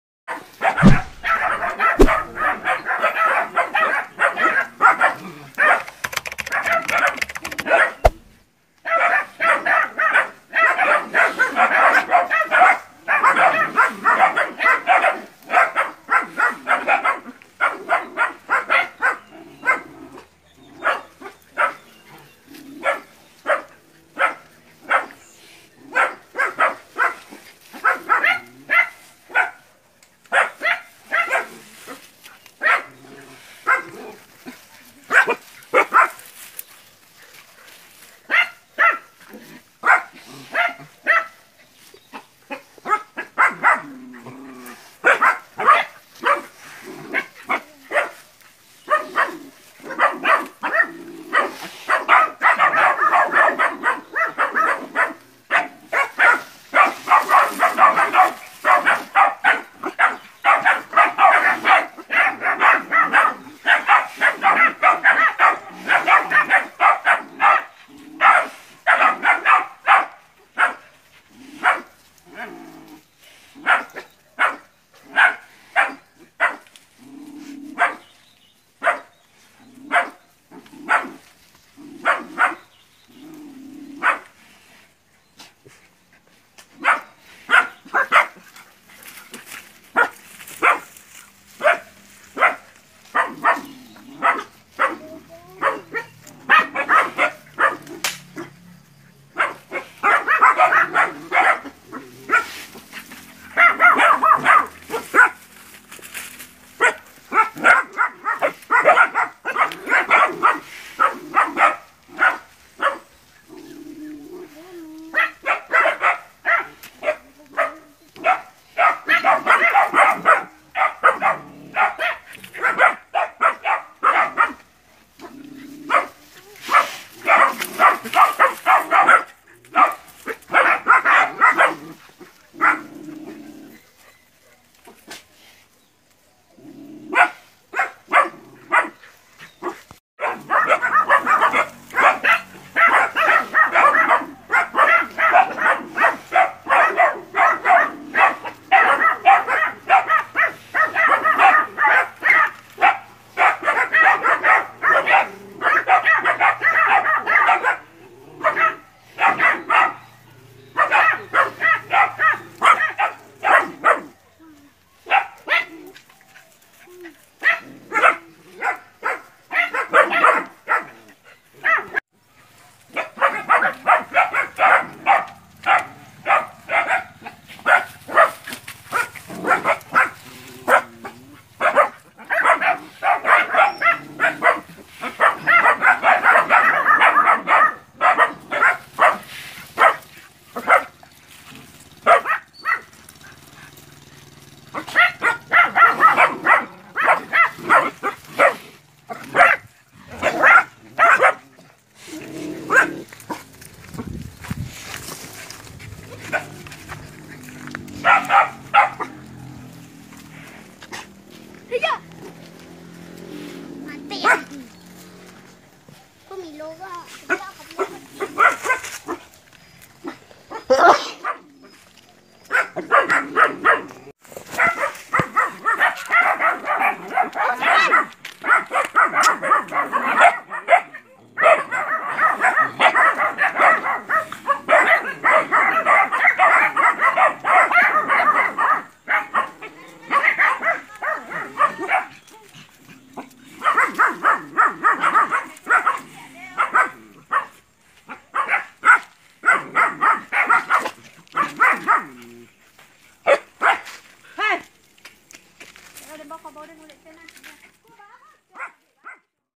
SALAH FOKUS !! ANAK ANJING VS KERBAU || AMUNISI 2020 PORBI SUMBAR
9 ekor anak anjing mengahadang kerbau besar,
Latihan sebelum terjun kelapangan 😁
Porbi sumbar, porbi marenten, porbi indonesia..